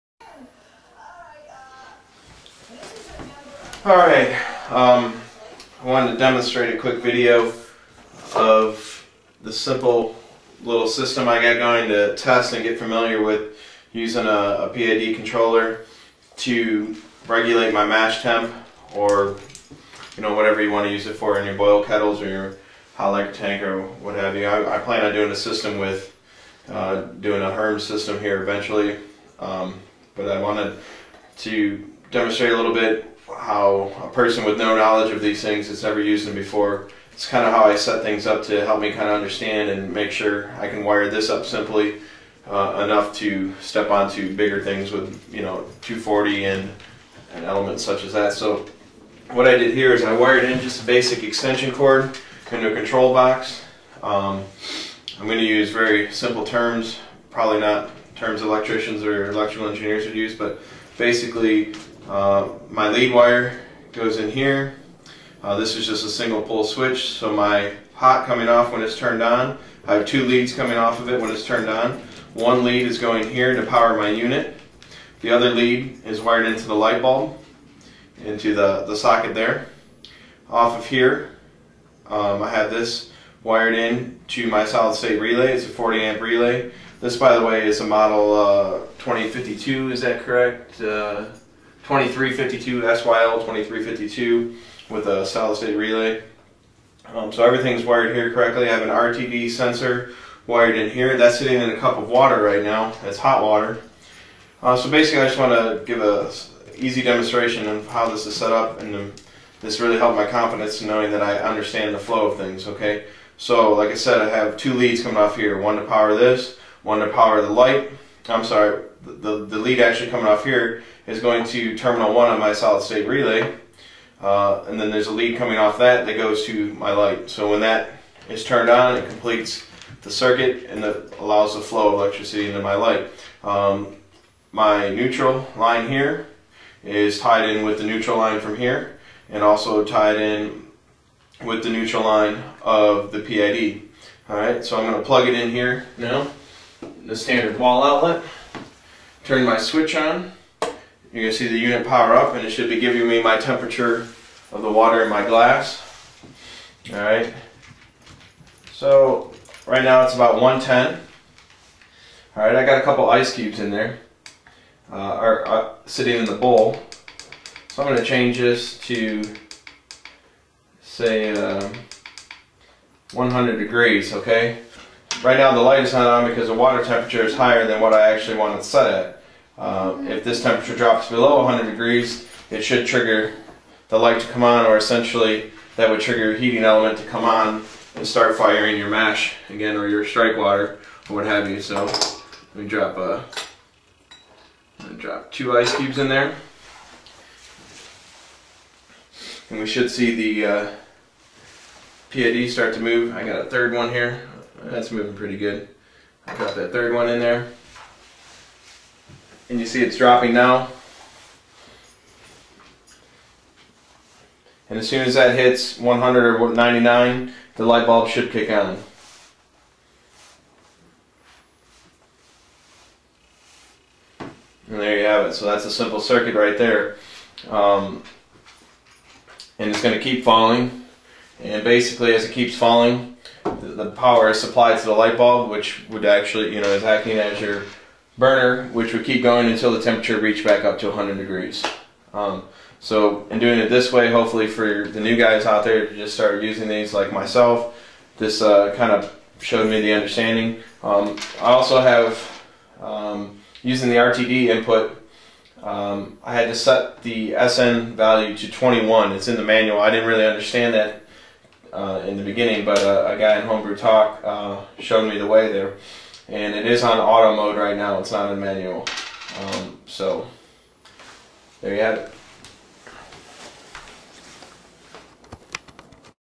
Simple PID Demonstration .mov
Just a simple hookup and demonstration using a Auber SYL-2352 PID Controller. I simulated the heating element by using a light bulb.